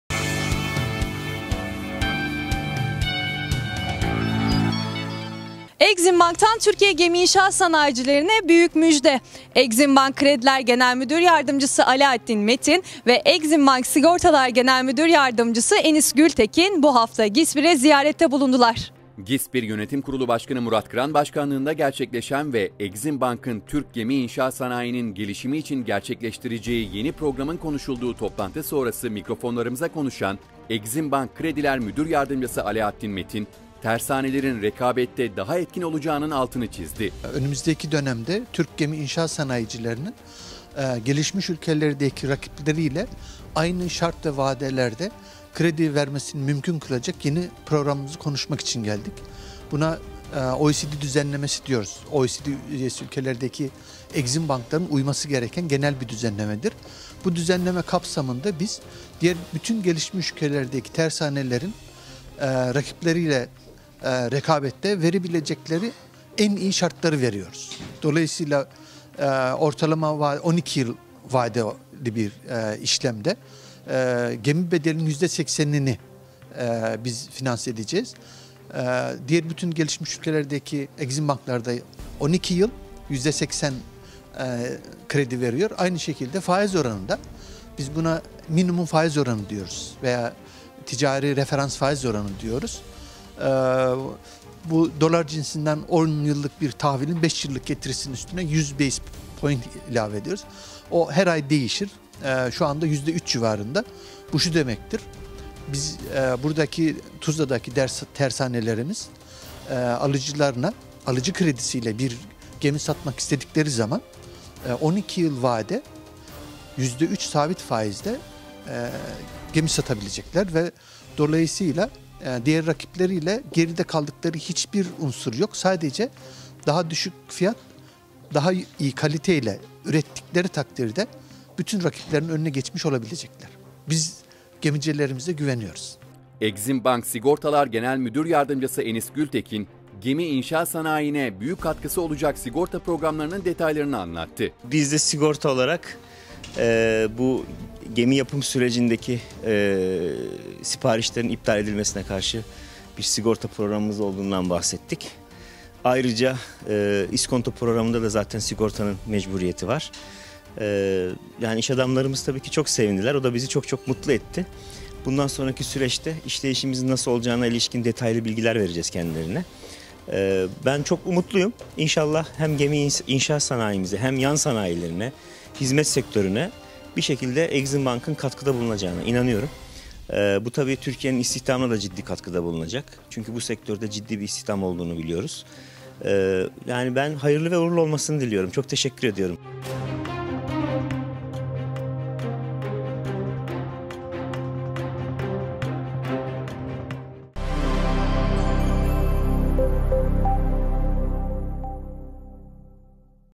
Türk Eximbank'dan Gemi İnşa Sanayi'ne müjde
GİSBİR Yönetim Kurulu Başkanı Murat Kıran Başkanlığı’nda gerçekleşen ve Türk Eximbank’ın Türk Gemi İnşa Sanayinin gelişimi için gerçekleştireceği yeni programın konuşulduğu toplantı sonrası mikrofonlarımıza konuşan Türk Eximbank Krediler Müdür Yardımcısı Alaattin Metin tersanelerin rekabette daha etkin olacağının altını çizdi.